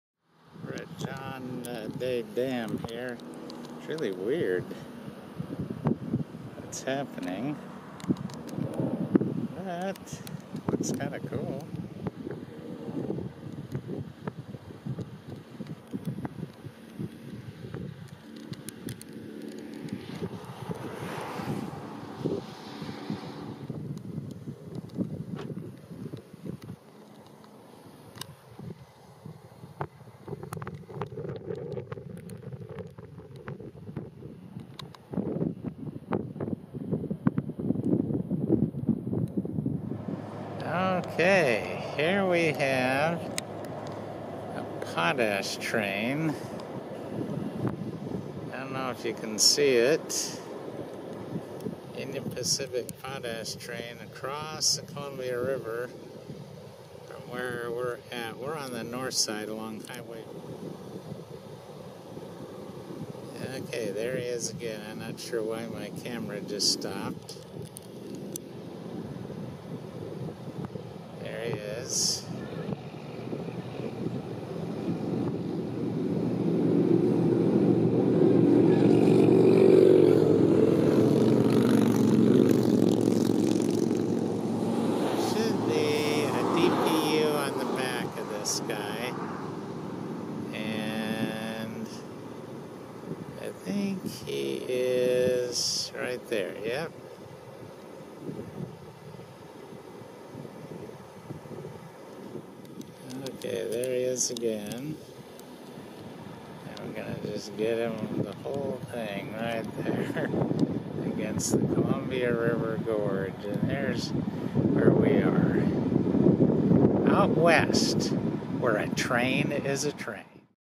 Columbia River Gorge east of Maryhill and westbound UP grain train
My wife and I like to travel WA SR 14 due to the many views the Columbia River and the fact that the Union Pacific RR (and I-84) occupy the south side and the BNSF Railway (and SR 14) occupy the north side. It also doesn't hurt that there is virtually no traffic on 14. Our first views are of John Day Dam looking east. It appears that three of the gates are open letting water through in a dramatic fashion. The remainder of this video shows a westbound Union Pacific grain train.